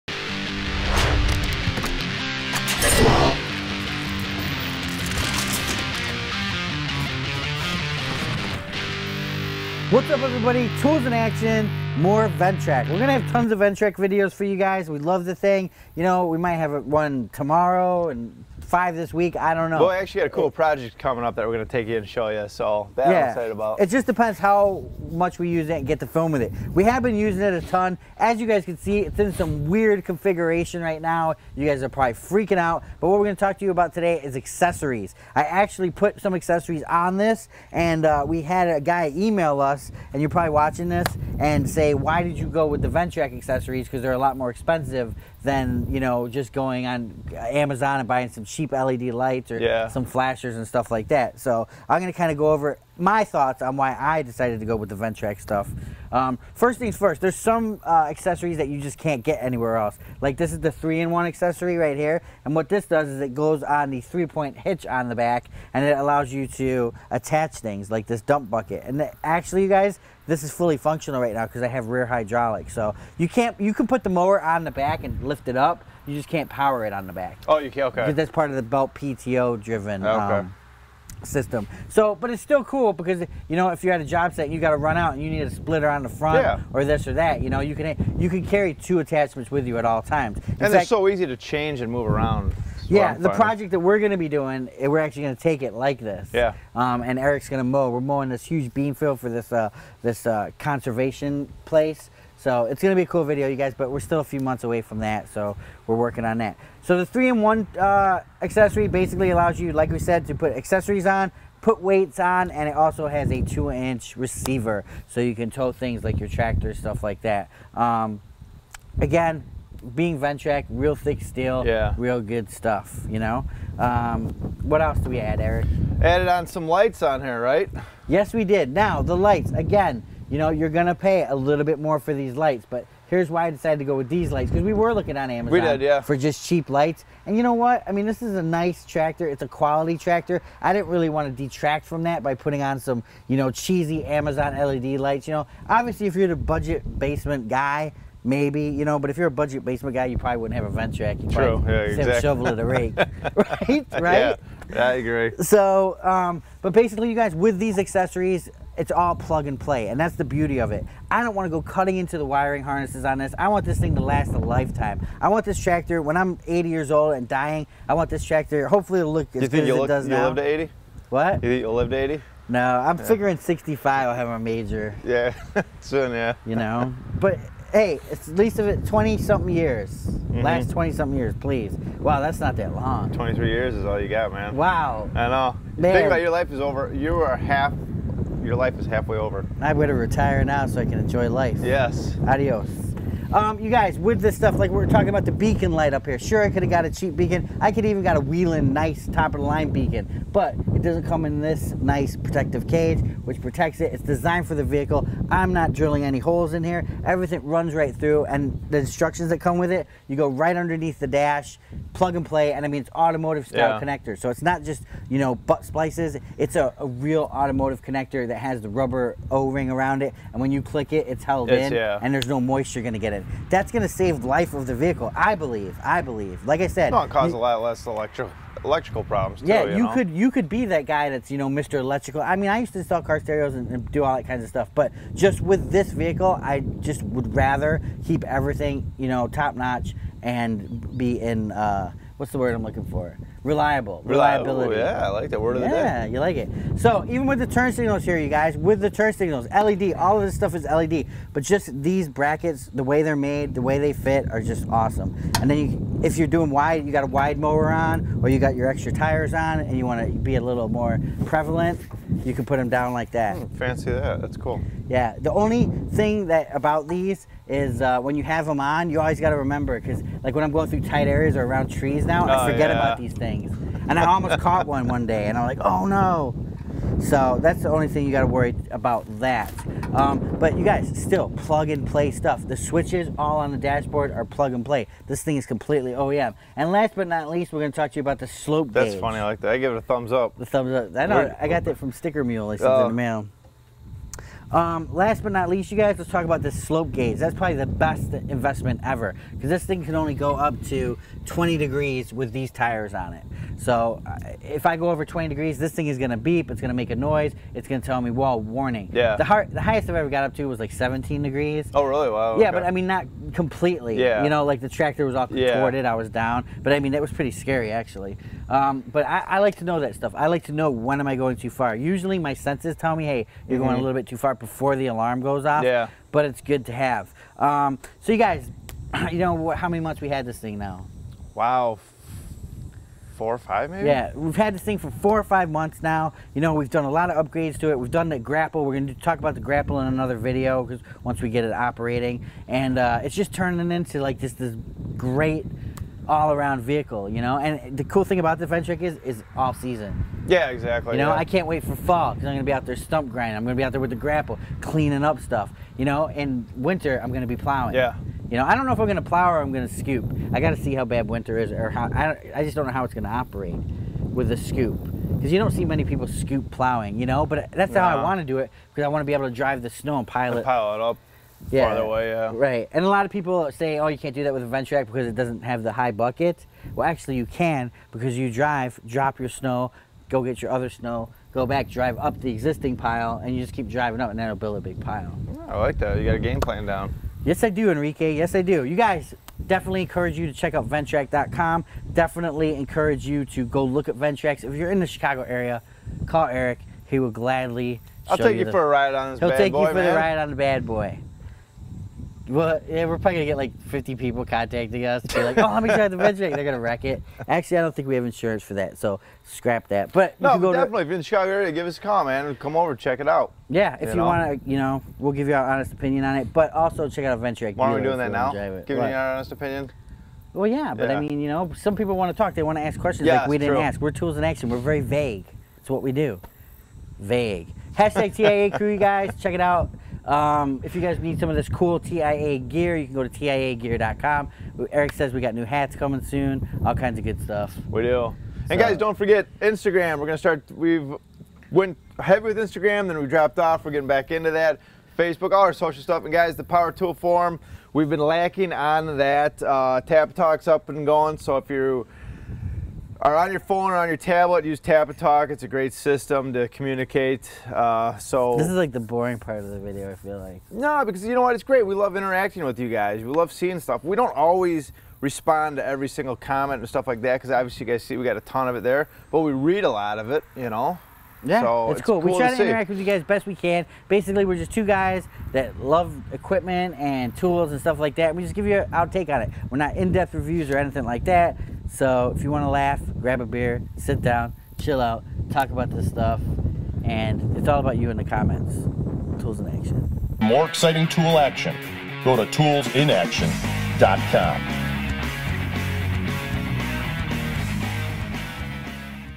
Ventrac Tractor Accessories
A reader emailed us and asked why we chose to use OEM accessories vs other alternatives.  We check out the turn signals, beacon light, three in one adapter and slope indicator.